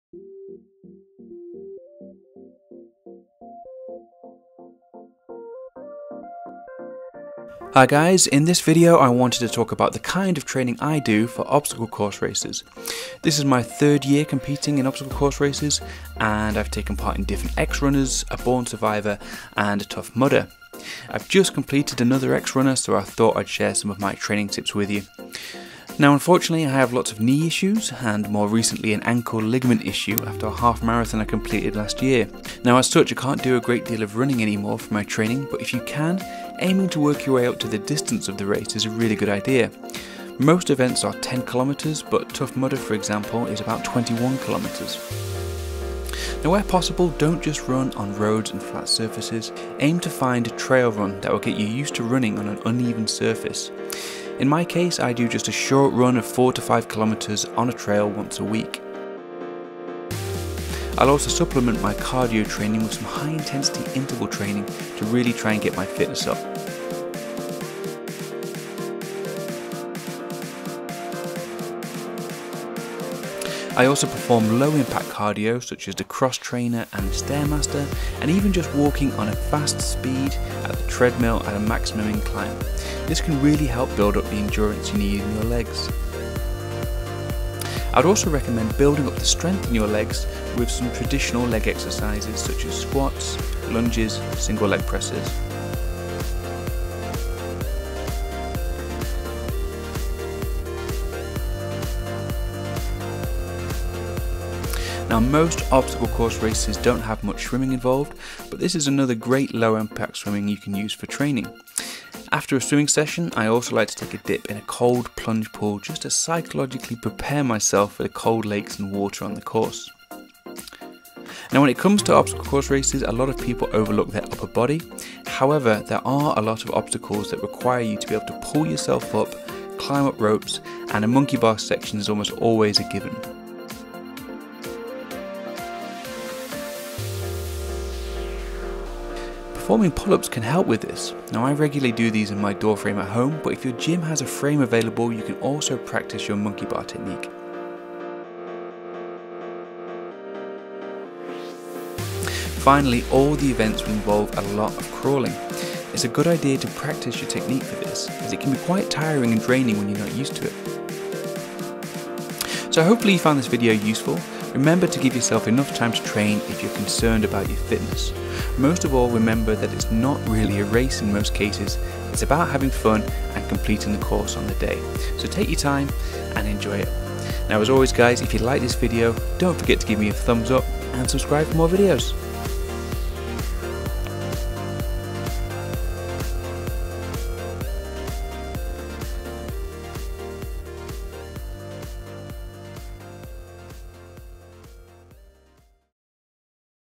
How to train for an obstacle course race (OCR) | Tough Mudder | X-Runner | Born Survivor | Spartan
This video is about how to train for an obstacle race (OCR), and the kind of training I do for obstacle course races. This is my third year of competing in obstacle race events, and I've taken part in different X-Runners, a Born Survivor and Tough Mudder. I have also recently completed another obstacle course race so I thought I'd share my training tips.

Now unfortunately I have lots of knee issues, and more recently an ankle ligament issue after the half marathon I completed last year. As such I can't do a great deal of running for my training, but if you can, aiming to work your way up to the distance of the race is a good idea. Most events are 10km, but tough Mudder is about 21km.

Where possible don't just run on roads and flat surfaces. Aim to find a trail run that will get you used to running an uneven surface. In my case I will do just a short run of around 4-5km on a trail once a week. I'll also supplement my cardio training with some high intensity interval training to really get my fitness up.
I also perform low impact cardio such as the cross trainer and the stair master, and even just walking at a fast speed on the treadmill at maximum incline. This can really help build up endurance in your legs.

I would also recommend building up strength in your legs with traditional leg exercises, such as squats, lunges and single leg presses.

Most obstacle course races don't have much swimming, but this is another great low impact exercise you can use for training. After a swimming session I also like to take a dip in the cold plunge pool to psychologically prepare myself for the cold lakes and water on the course.

Now when it comes to obstacle course races, a lot of people overlook their upper body. However there are a lot of obstacles that require you to be able to pull yourself up, climb up ropes and a monkey bar section is almost always a given. Performing pull ups can help with this; I do these regularly just in my door frame at home, but if your gym has a frame available you can also practice your monkey bar technique.

Finally, all the events will involve a lot of crawling. It's a good idea to practice your technique for this as it can be quite tiring and draining when you're not used to it.

So hopefully you found this video useful. Remember to give yourself enough time to train especially if your concerned about your fitness. Most of all, remember that it's not really a race in most cases - it's about having fun and completing the course on the day, so take your time and enjoy it!